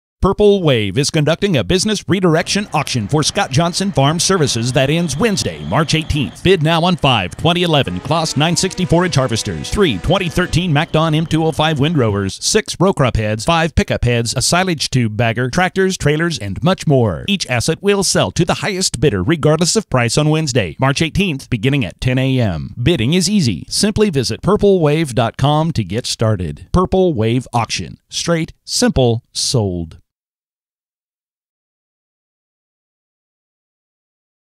Scott Johnson Farm Services business redirection auction | March 18, 2015 | Purple Wave
Scott Johnson Farm Services business redirection auction
March 18, 2015

84 items in this absolute auction will begin to close at 10 a.m. CDT on Wednesday, Mar. 18. The auction includes 84 items in TX. Categories currently include tractors, hay and forage, grain handling, trailers, harvesting, air compressors, tractor loaders and attachments, heavy trucks, automotive, welders and welding tools, tools, automobiles, utility and service trucks - view and bid on all items

Purple Wave is conducting an business redirection auction for Scott Johnson Farm Services that ends Wednesday, March 18. Bid now on five 2011 Claas 960 forage harvesters, three 2013 MacDon M205 windrowers, six row crop heads, five pickup heads, a silage tube bagger,  tractors, trailers and much more. Each asset will sell to the highest bidder regardless of price on Wednesday, March 18, beginning at 10 a.m. Bidding is easy - simply visit purplewave.com to get started. Purple Wave Auction. Straight. Simple. Sold.